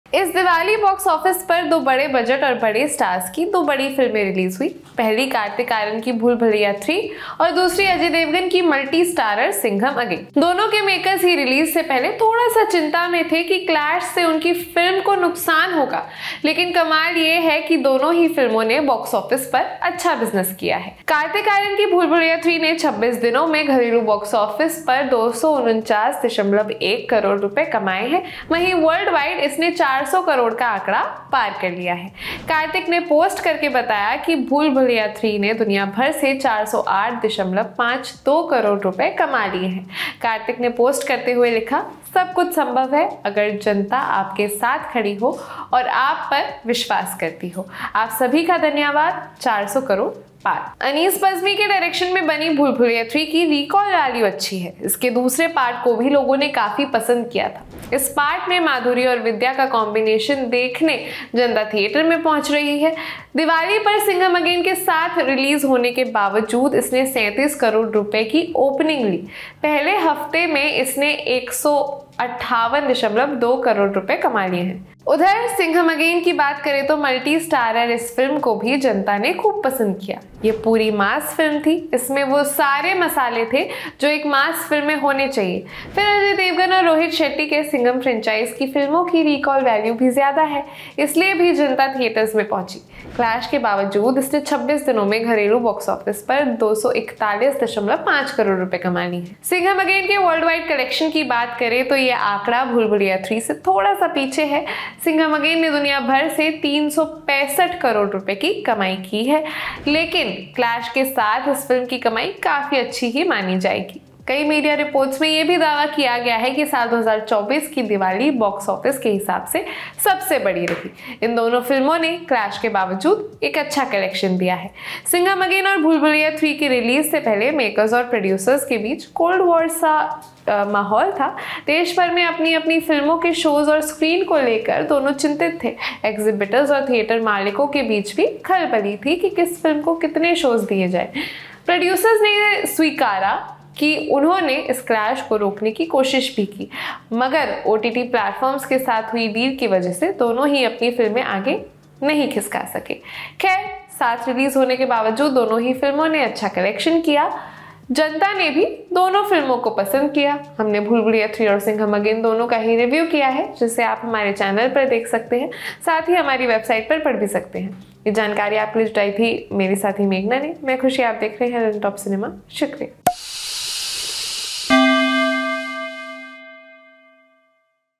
Bhool Bhulaiyaa 3 और Singham Again के Box Office Collection में कौन किससे आगे? Kartik Aaryan | Ajay
Kartik Aaryan's Bhool Bhulaiyaa 3 and Ajay Devgan and Rohit Shetty's Singham Again released together on 1st November, 2024. Bhool Bhulaiyaa 3 have crossed the 400-crore mark. What did Kartik Aaryan say about it, know through this video.